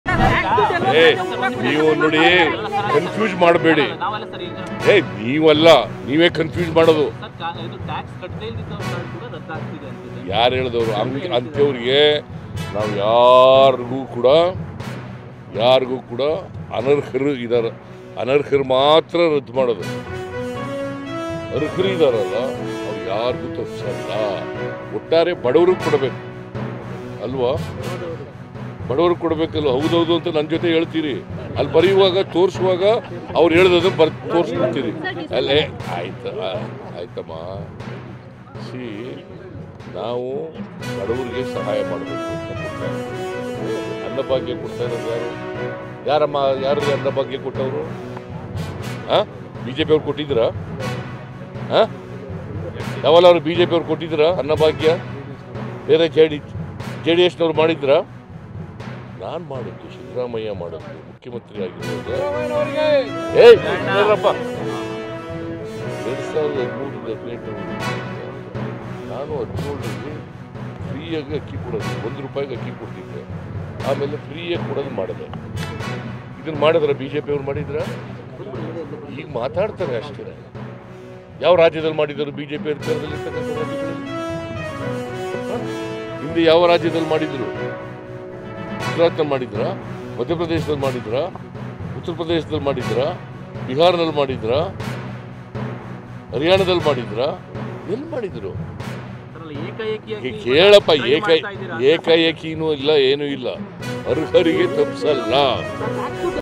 Ration card not cancelled! : ರೇಷನ್ ಕಾರ್ಡ್ ರದ್ದು ಮಾಡಿಲ್ಲ! | H16 News Kannada | Viral News Kannada
ಅನರ್ಹ ಬಿಪಿಎಲ್ ಕಾರ್ಡ್ ಮಾತ್ರ ರದ್ದು!
ಕನ್ಫ್ಯೂಸ್ ಮಾಡುತ್ತಿರುವ ಬಿಜೆಪಿ!
ರೇಷನ್ ಕಾರ್ಡ್ ರದ್ದು ಮಾಡಿಲ್ಲ!

Karnataka | ಕರ್ನಾಟಕ
Kannada | ಕನ್ನಡ